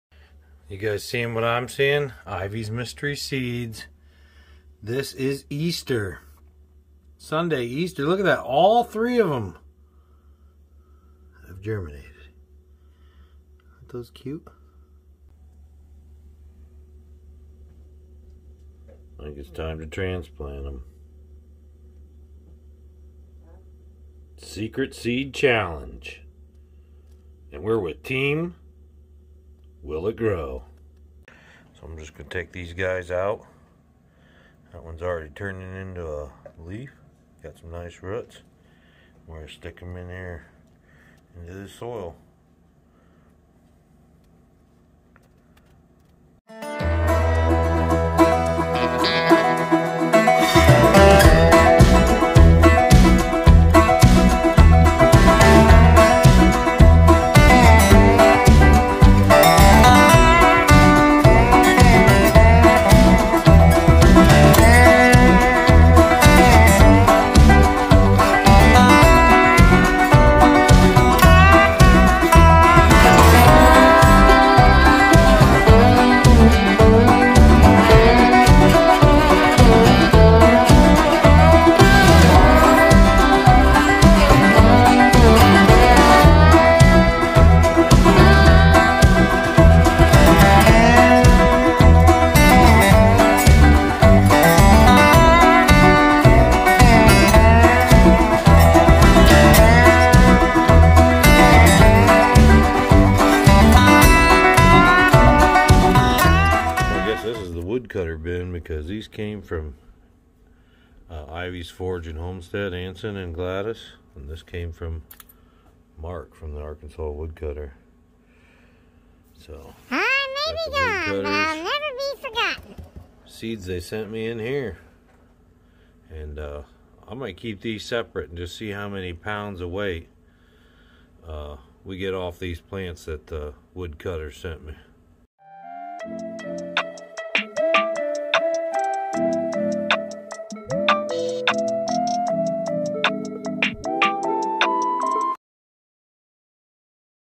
SECRET SEED, Germinated and in the soil
So the "Secret Seed" s that Ivey's Forge and Homestead sent us have germinated and we planted them in the soil. Let's Check it out!